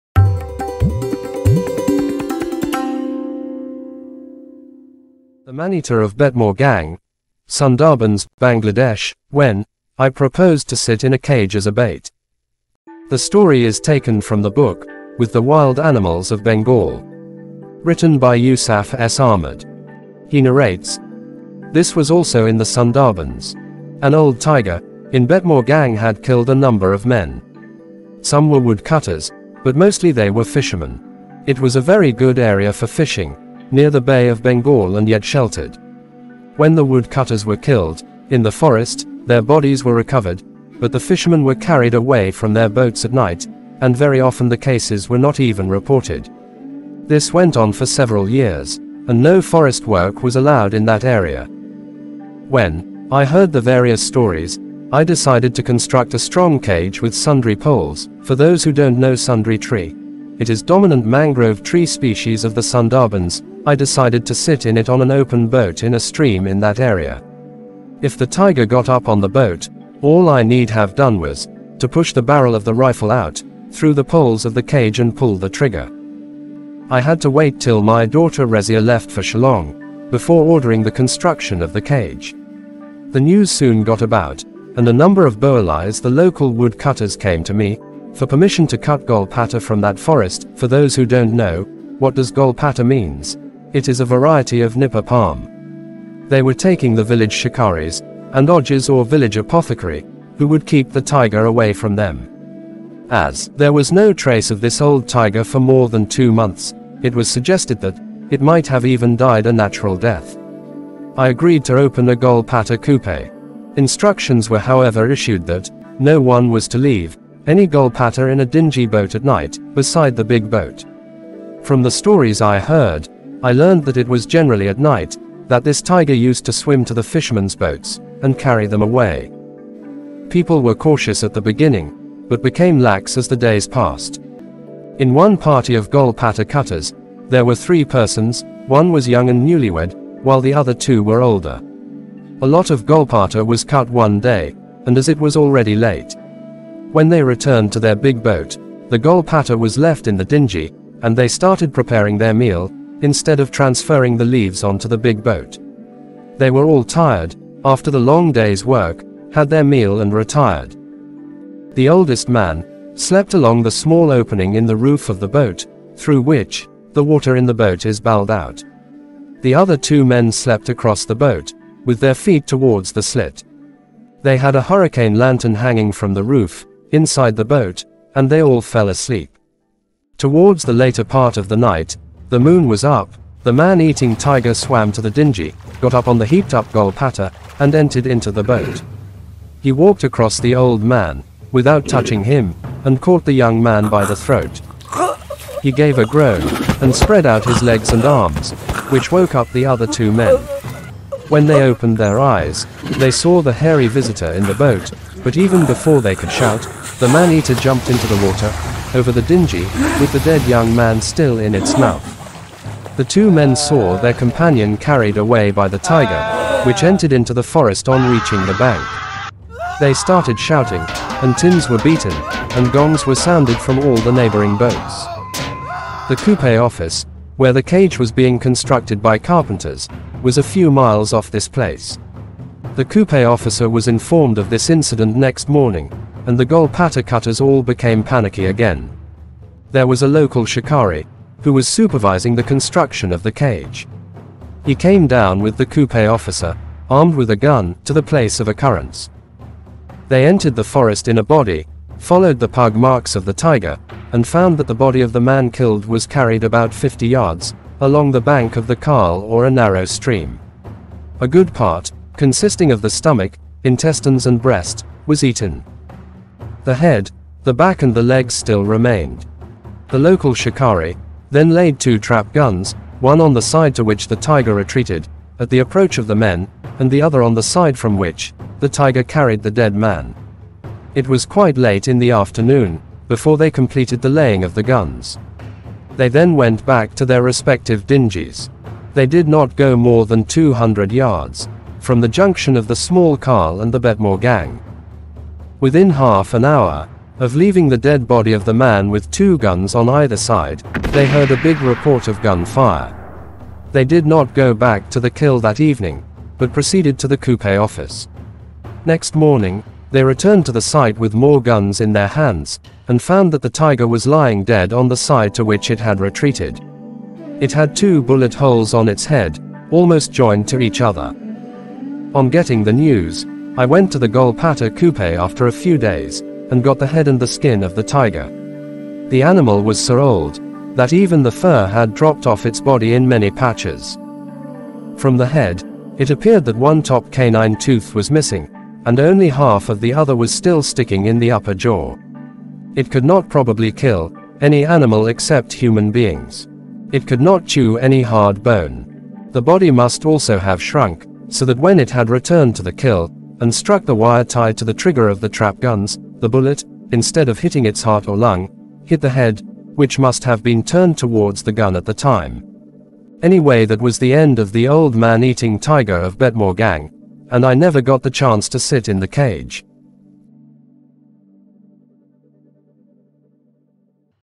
The Man-Eater Of Betmorgang When Sir Yusuf S. Ahmed Proposed to Sit In A Cage As A Bait (English)
The story is taken from the Book “With the wild animals of Bengal”. Written by Yousaf S. Ahmad;